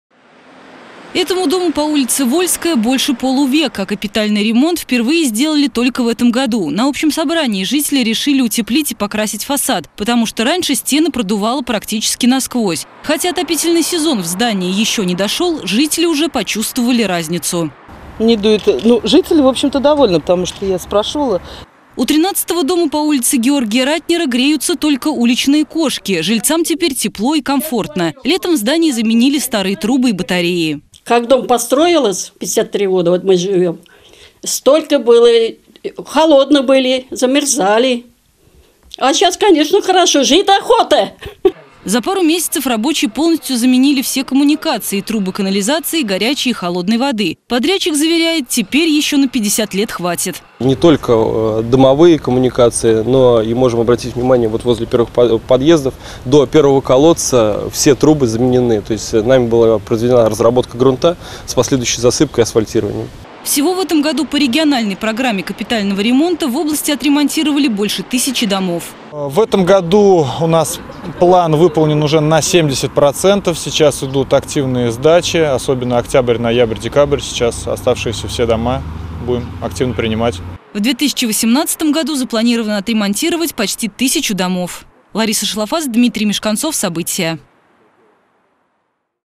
2500 многоквартирных домов отремонтировали в регионе за весь период действия программы капремонта
2500 многоквартирных домов отремонтировали в регионе за весь период действия программы кап ремонта. Что нужно привести в порядок в первую очередь - крышу, коммуникации или фасад решают жители.